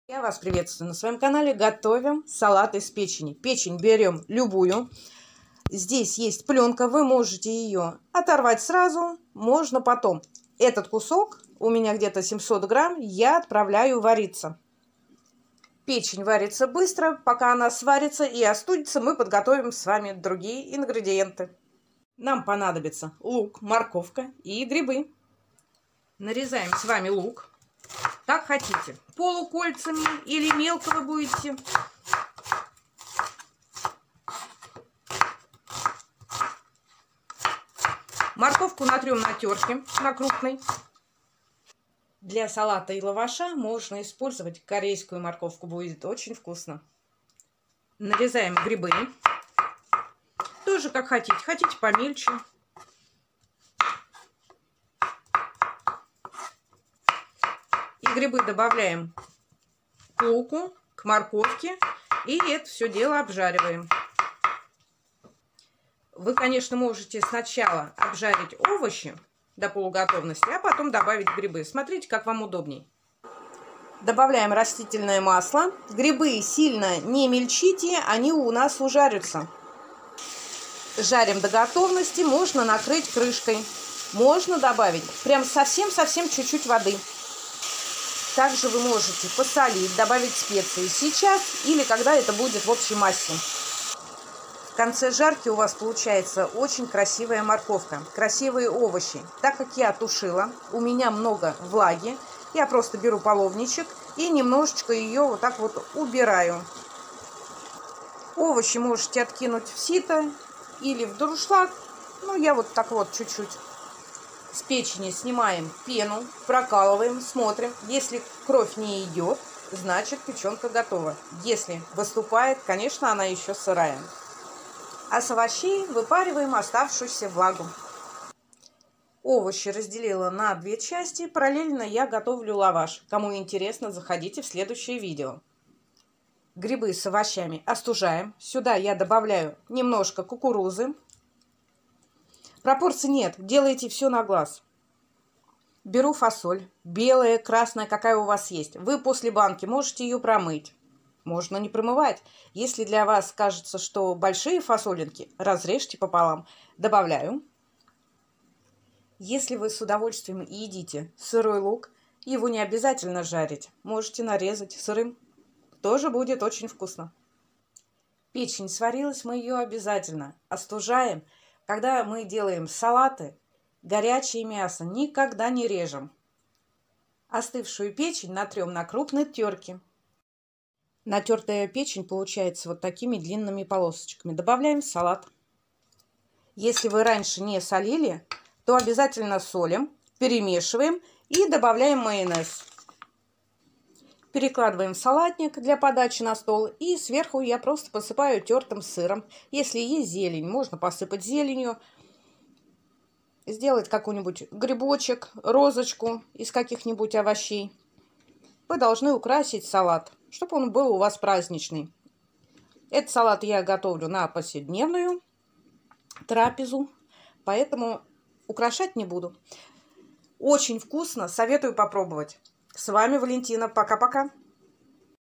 Салат из печени.
Простой салат из печени.
Вкусный, полезный и праздничный.
Грибы, овощи пожарить или потушить. Печень сварить, остудить натереть на крупной тёрке.
Овощи, печень, кукурузу, фасоль соль, специи, зелень соединить и перемешать добавить майонез и посыпать сыром. Салат готов.
Приятного аппетита.